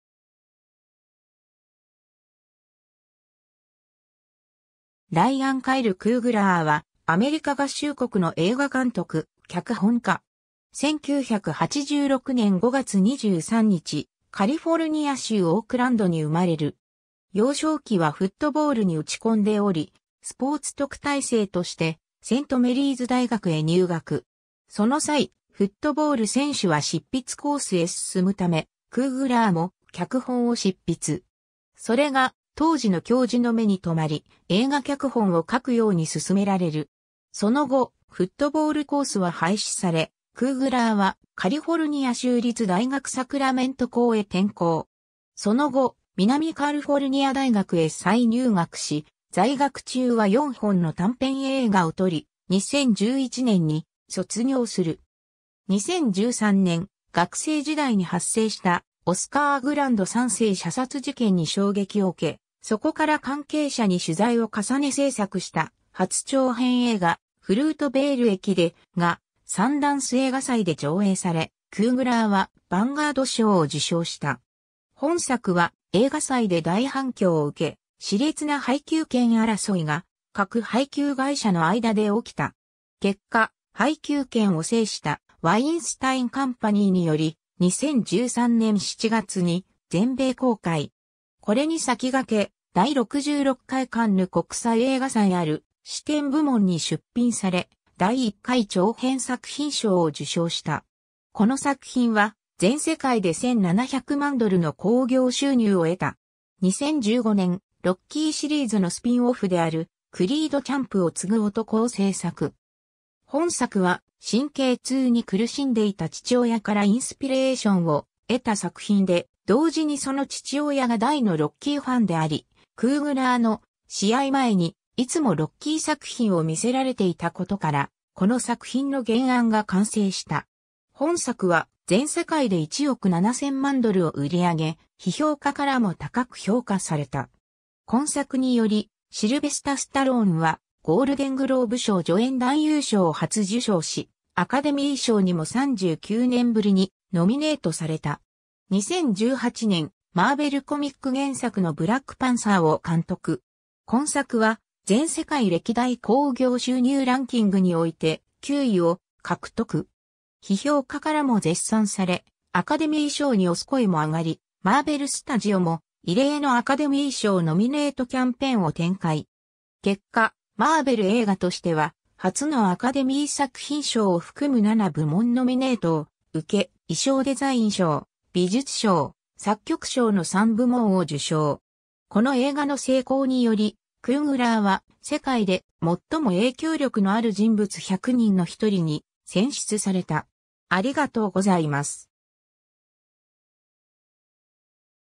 ライアン・クーグラー
ライアン・カイル・クーグラー（Ryan Kyle Coogler、1986年5月23日 - ） は、アメリカ合衆国の映画監督、脚本家。
 1986年5月23日、カリフォルニア州オークランドに生まれる。
幼少期はフットボールに打ち込んでおり、スポーツ特待生として、セント・メリーズ大学へ入学。
その際、フットボール選手は執筆コースへ進むため、クーグラーも脚本を執筆。
それが当時の教授の目に留まり、映画脚本を書くように奨められる。
その後、フットボールコースは廃止され、クーグラーはカリフォルニア州立大学サクラメント校へ転校。
その後、南カルフォルニア大学へ再入学し、在学中は4本の短編映画を撮り、2011年に卒業する。
 2013年、学生時代に発生したオスカー・グランド三世射殺事件に衝撃を受け、そこから関係者に取材を重ね制作した初長編映画、『フルートベール駅で』がサンダンス映画祭で上映され、クーグラーはヴァンガード賞を受賞した。
本作は映画祭で大反響を受け、熾烈な配給権争いが各配給会社の間で起きた。
これに先駆け、第66回カンヌ国際映画祭ある視点部門に出品され、第一回長編作品賞を受賞した。
この作品は全世界で1700万ドルの興行収入を得た。
 2015年、ロッキーシリーズのスピンオフである『クリード チャンプを継ぐ男』を制作。
本作は神経痛に苦しんでいた父親からインスピレーションを得た作品で、同時にその父親が大のロッキーファンであり、クーグラーの試合前に、いつもロッキー作品を見せられていたことから、この作品の原案が完成した。
本作は、全世界で1億7千万ドルを売り上げ、批評家からも高く評価された。
今作によりシルベスタ・スタローンはゴールデングローブ賞 助演男優賞を初受賞し、アカデミー賞にも39年ぶりにノミネートされた。
 2018年、マーベル・コミック原作のブラックパンサーを監督。
今作は全世界歴代興行収入ランキングにおいて、9位（当時）を獲得。
批評家からも絶賛され、アカデミー賞に推す声も上がり、マーベル・スタジオも異例のアカデミー賞ノミネートキャンペーンを展開。
結果、マーベル映画としては初のアカデミー作品賞を含む7部門ノミネートを受け、衣装デザイン賞、美術賞、作曲賞の三部門を受賞。
この映画の成功により、クーグラーは世界で最も影響力のある人物100人の一人に選出された。